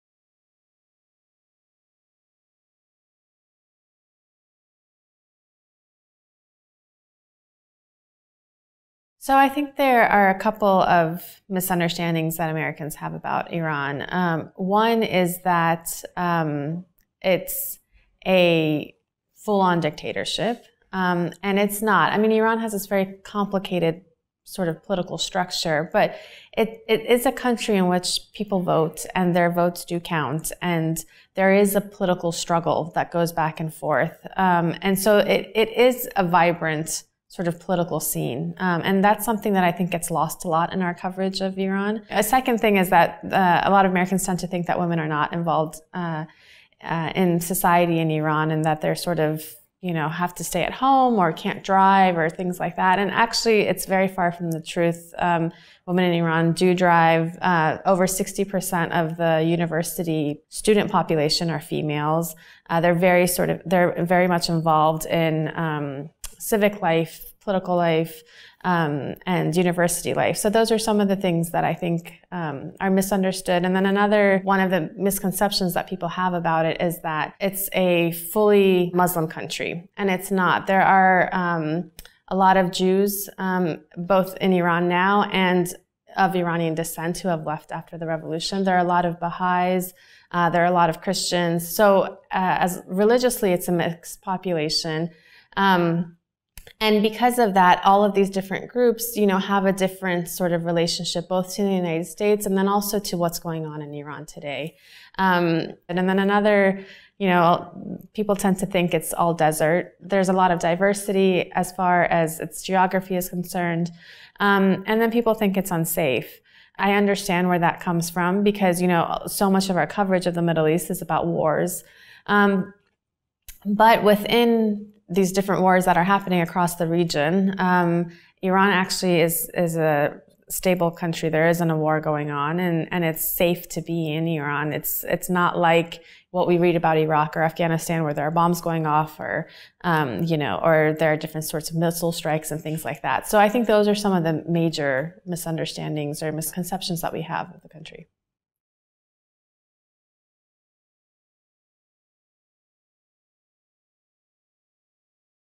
What are some misunderstandings that people in the U.S. have about Iran?
Narges Bajoghli, Johns Hopkins University.
This video is part of the following Choices curriculum: The Iranian Revolution